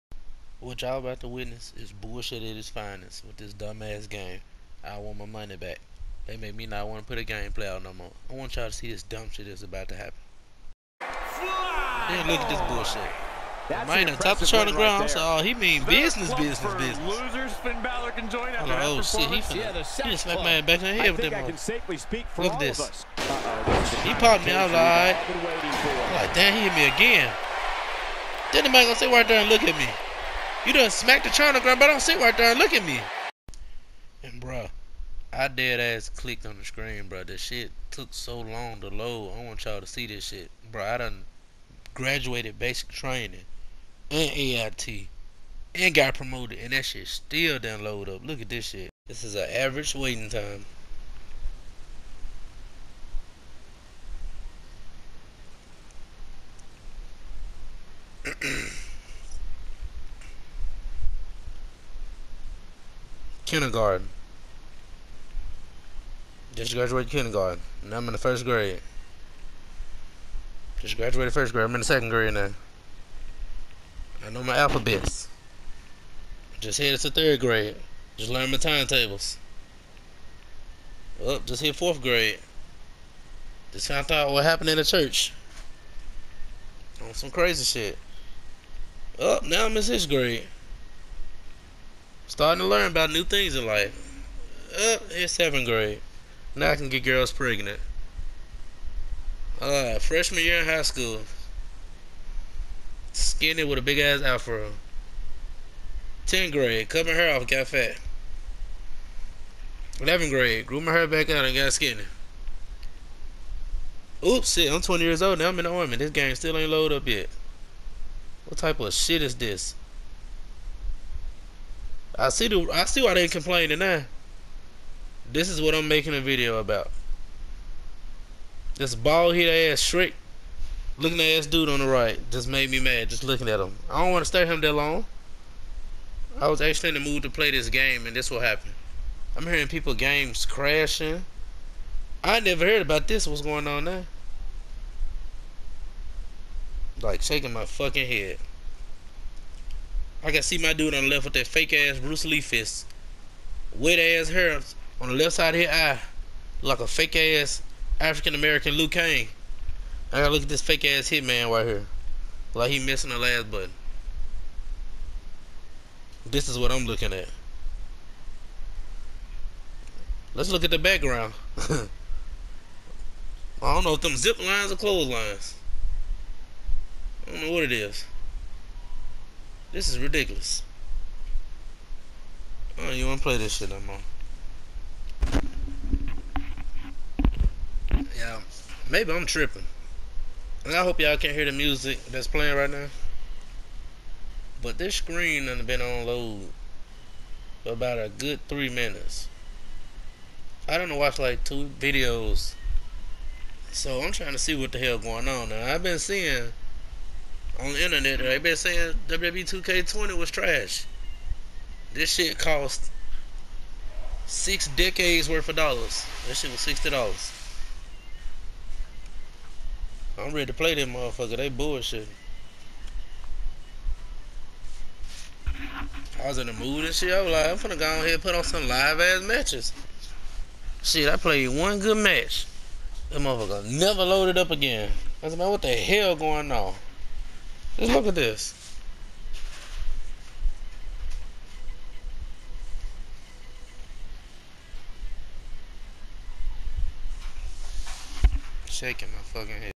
WWE 2K20 MUST BE STOPPED !!!
WWE 2K20 MUST BE STOPPED !!! FixWWE2K20. Give me your opinion on this game because I can't even play it now smh.